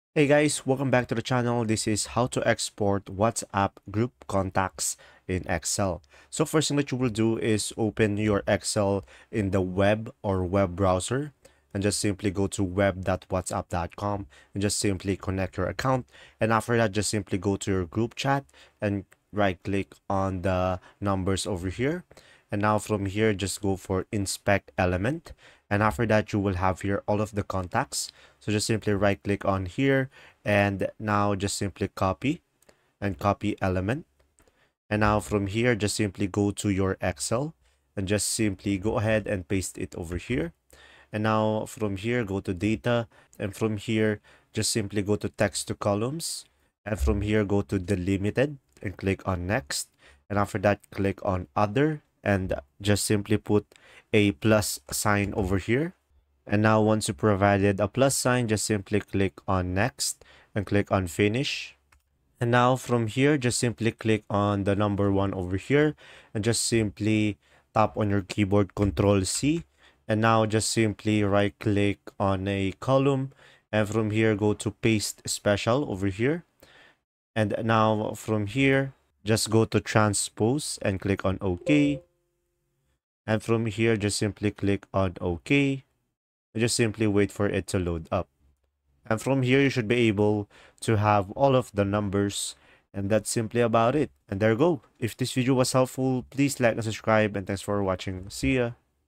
How to export Whatsapp group contacts in Excel (Step By Step) 2025
How to export Whatsapp group contacts in Excel (Step By Step)

Learn how to export Whatsapp group contacts in Excel with this step by step tutorial. Easily download and organize all your contacts for better management.